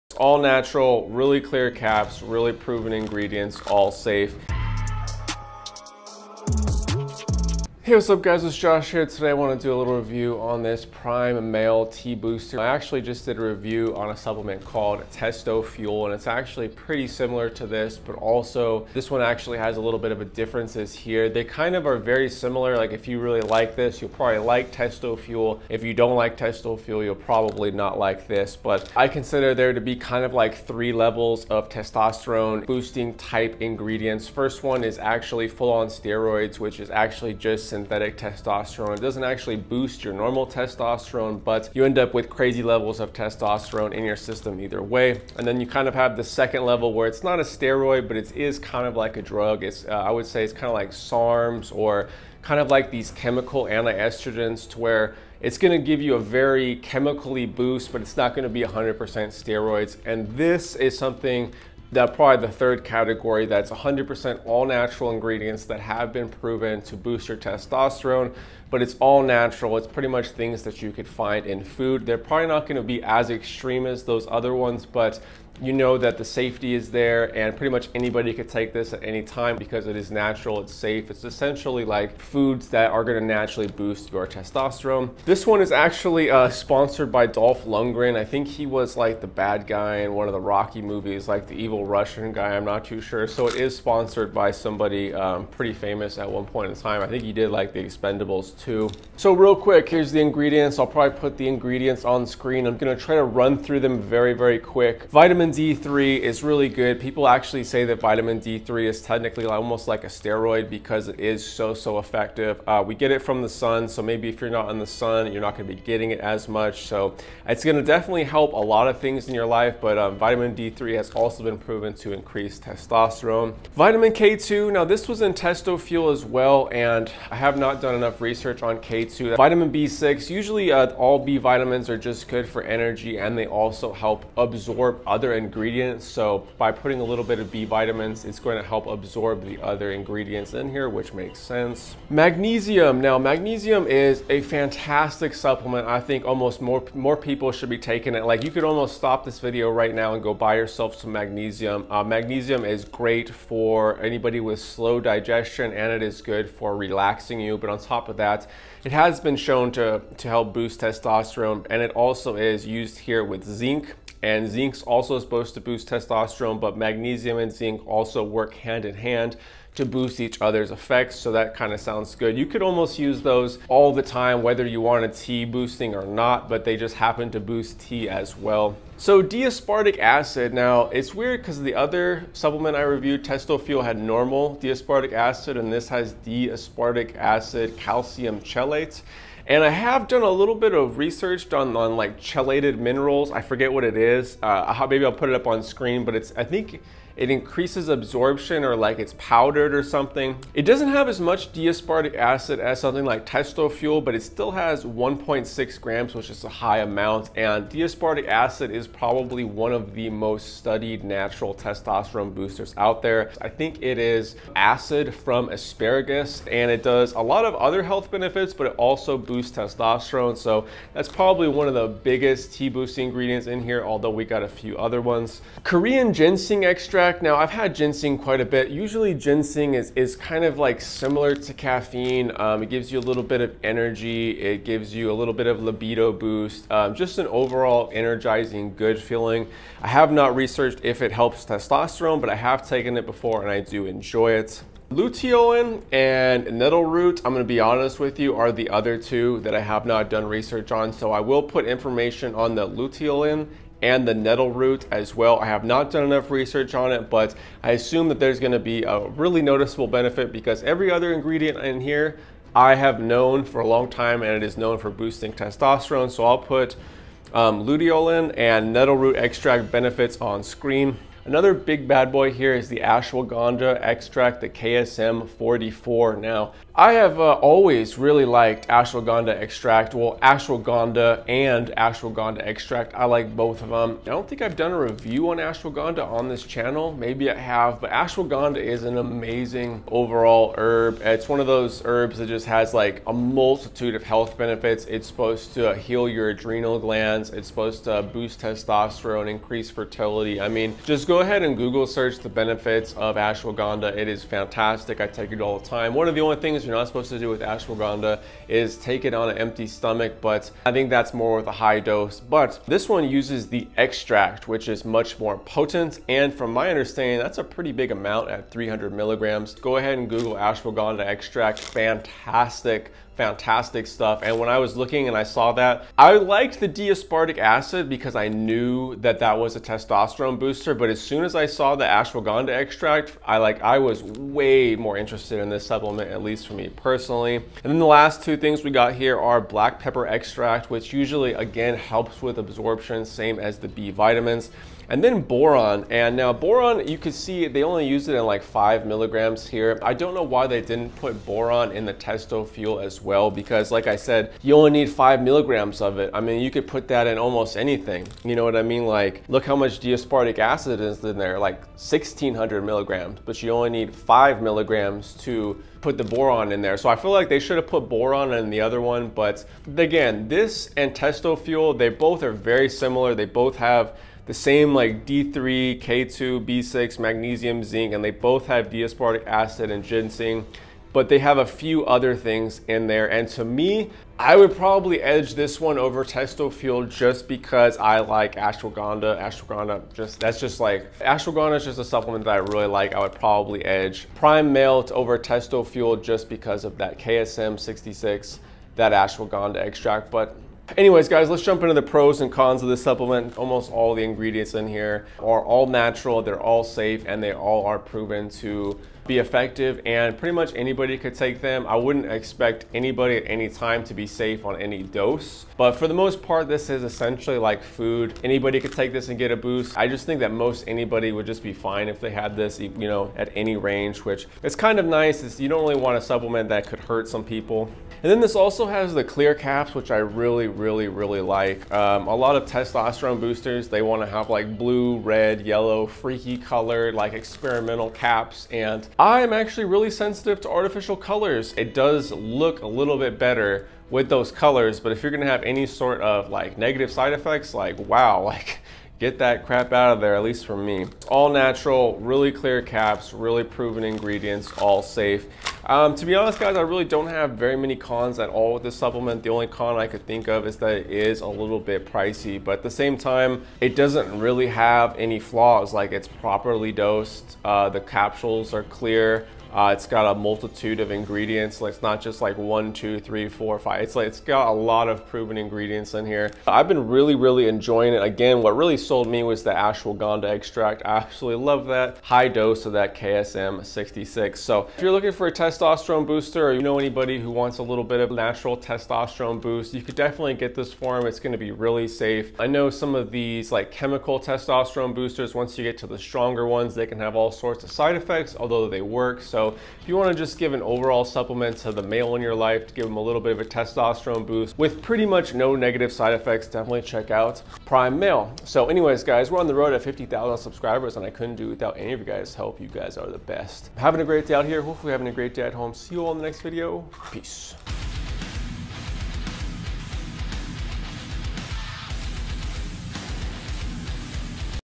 Prime Male Vitality (Strongest Test Booster 2022?)
Hello Friends! Today I wanted to share my thoughts on Prime Male Vitality complex, a great mix of all-natural potent ingredients. Have YOU tried any of these ingredients or this mix?

Send Me Anything! Address Below 👇
RealJoshTv
United States